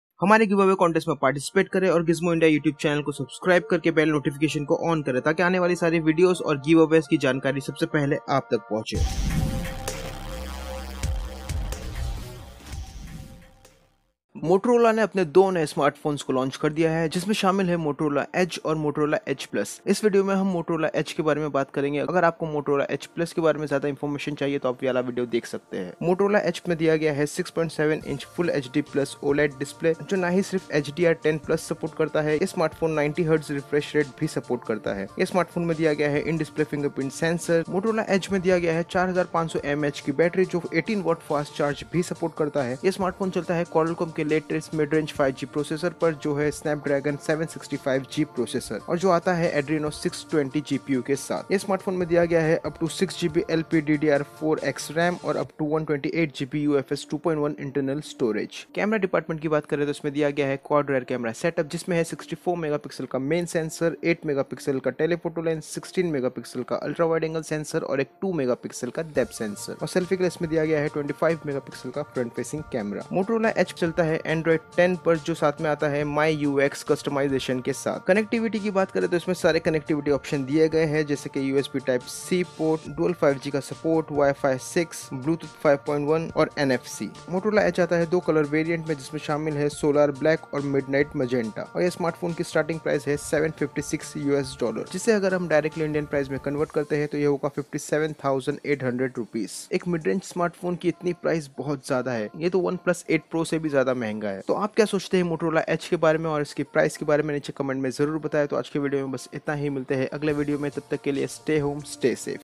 Motorola edge Launched | Price In India & Specifications
This is Gizmo India Youtube channel and you are watching Motorola Edge The Mid Range 5G Smartphone detail video.